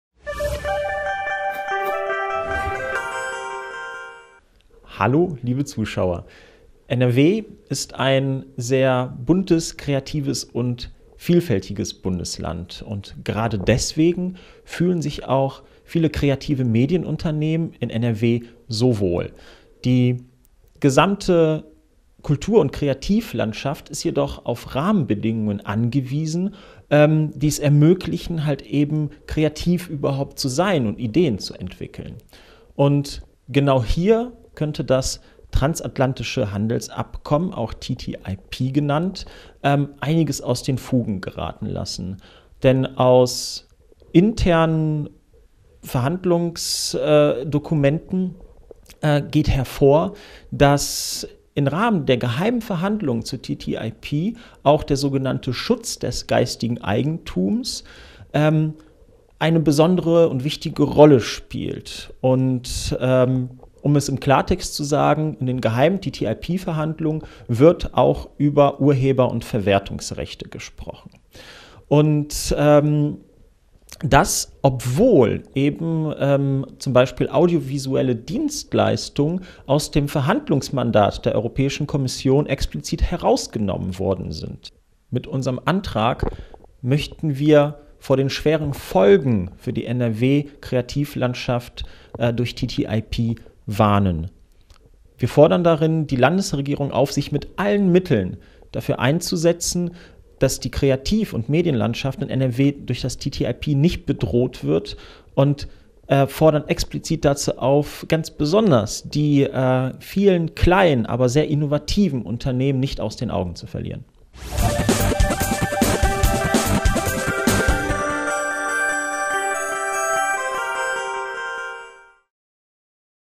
Lukas Lamla (@maltis) zu Gefahren von TTIP für kulturelle Vielfalt in NRW
Vom transatlantischen Freihandelsabkommen TTIP geht die Gefahr aus, die kulturelle Vielfalt in NRW zu  gefährden, z.B. über die Zementierung des innovationshemmenden Urheber- und Verwertungsrechts! Die Landesregierung muss die Kreativ- und Kulturlandschaft in NRW effektiv schützen!

Antrag: